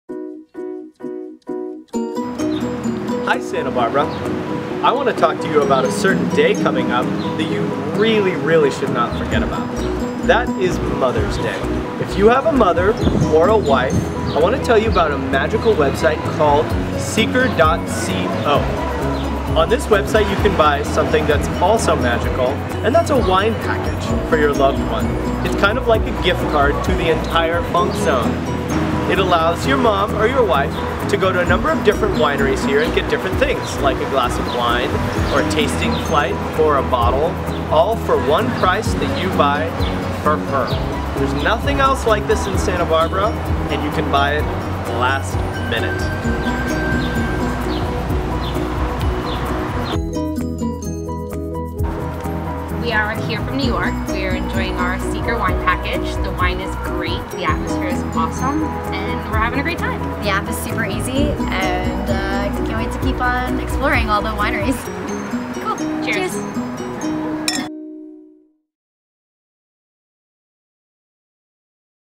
Seeker Wine Packages for Mother's Day
If you live near Santa Barbara, and your mother or wife likes wine, this is your gift!  The only multi-winery experience in the Funk Zone offers an afternoon or weekend of fun.  It's the only gift like it in SB!